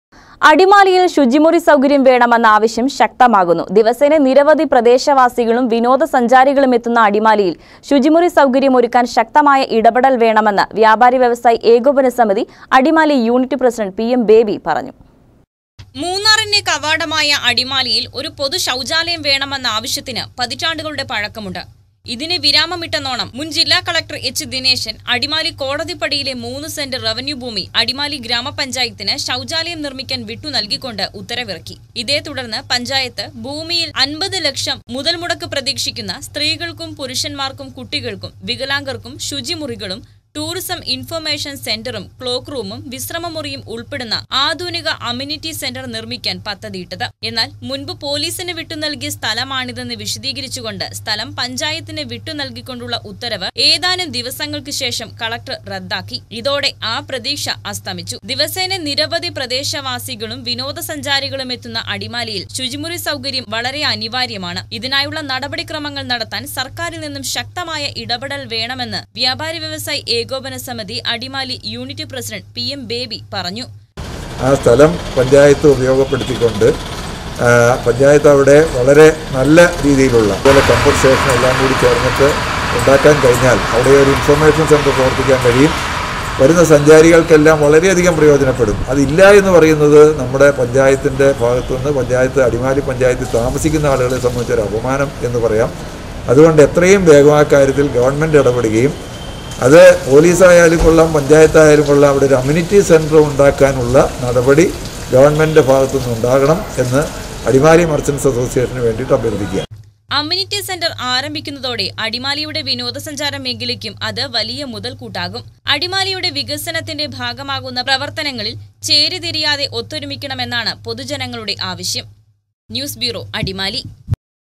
അടിമാലിയില്‍ ശുചിമുറി സൗകര്യം വേണമെന്ന ആവശ്യം ശക്തമാകുന്നു.
അടിമാലിയില്‍ ശുചിമുറി സൗകര്യം വേണമെന്ന ആവശ്യം ശക്തമാകുന്നു.  ദിവസേന നിരവധി പ്രദേശവാസികളും വിനോദ സഞ്ചാരികളും  എത്തുന്ന അടിമാലിയില്‍ ശുചിമുറി സൗകര്യം ഒരുക്കാന്‍ ശക്തമായ ഇടപെടല്‍ വേണമെന്ന് വ്യാപാരി വ്യവസായി ഏകോപന സമിതി അടിമാലി യൂണിറ്റ് പ്രസിഡന്റ് പി.എം ബേബി പറഞ്ഞു.